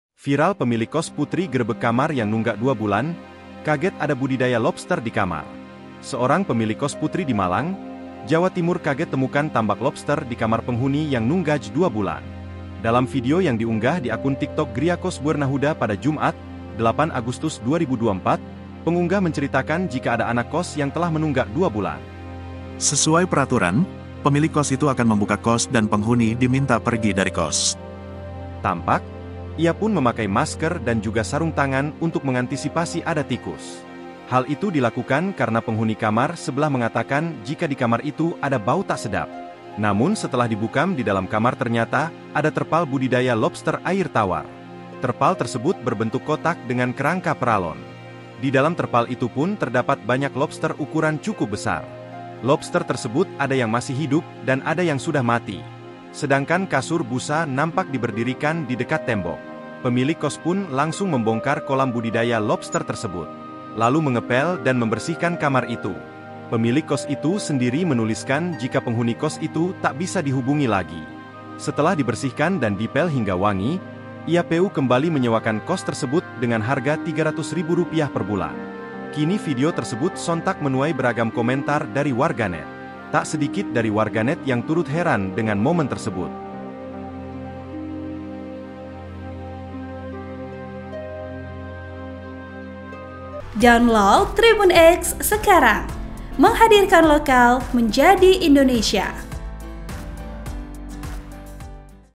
Viral Pemilik Kos Putri Gerebek Kamar yang Nunggak 2 Bulan, Kaget Ada Budidaya Lobster di Kamar
Seorang pemilik kos putri di Malang, Jawa Timur kaget temukan tambak lobster di kamar penghuni yang nunggak 2 bulan.

Dalam video yang diunggah di akun Tiktok @griyakosbuernahuda pada Jumat (8/8/2024), pengunggah menceritakan jika ada anak kos yang telah menunggak dua bulan.

Sesuai peraturan, pemilik kos itu akan membuka kos dan penghuni diminta pergi dari kos.

Program:
Host:
Editor Video: Mayang Putri Anugraha
Uploader: